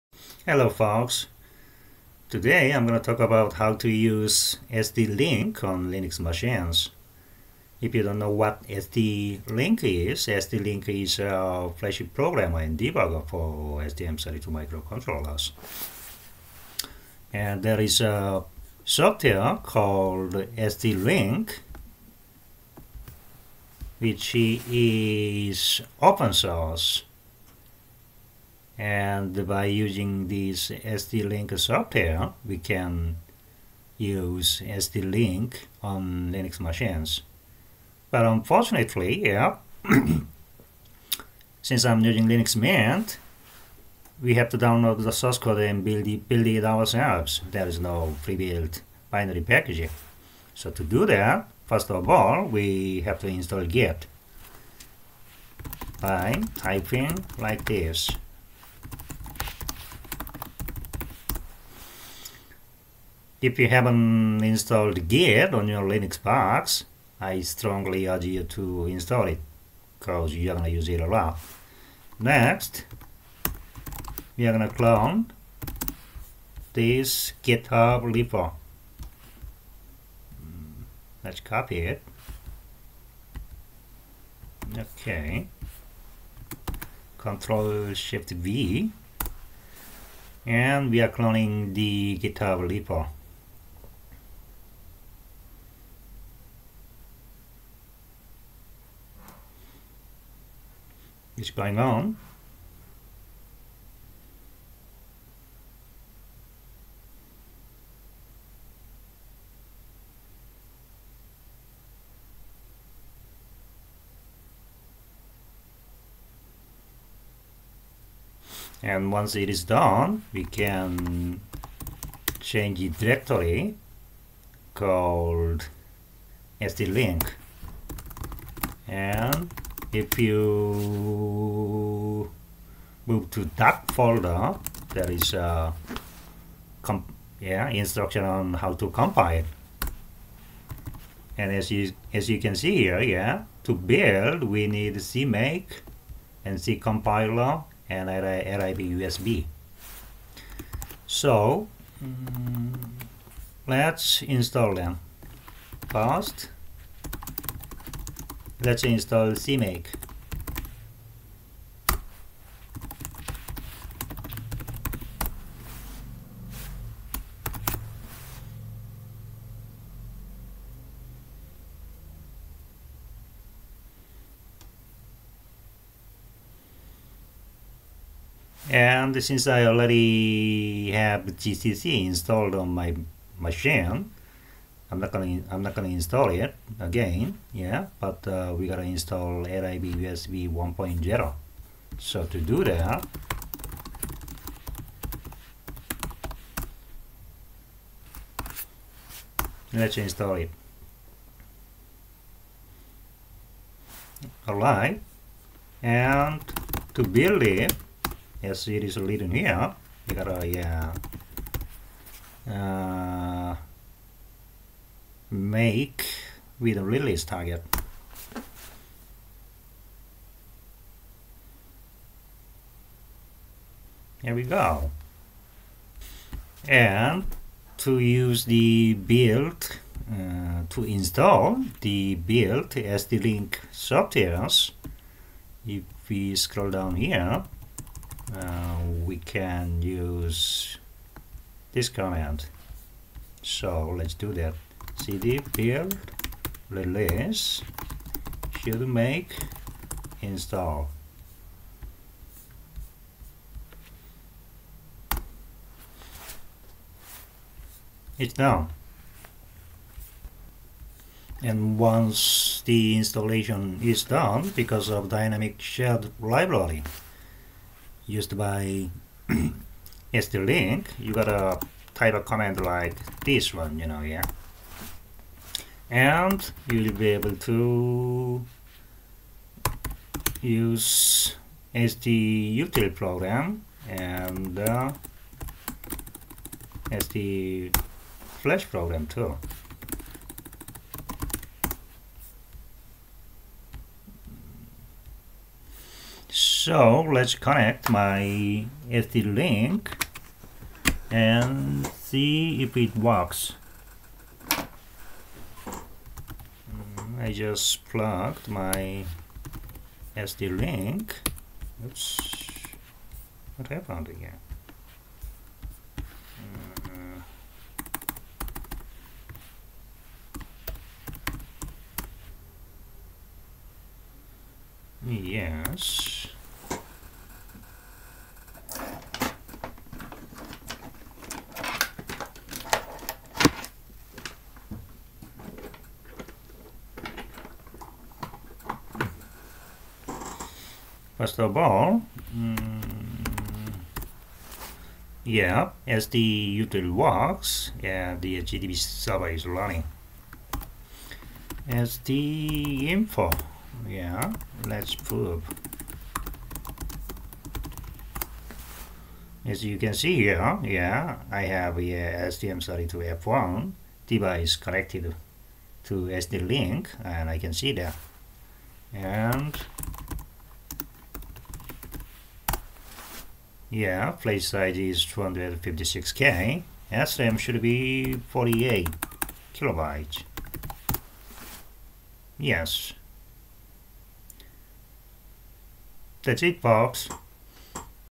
stm32 development on Linux - installing stlink
this video describes how to install stlink open source software on Debian based Linux machine. I'm personally using Linux Mint.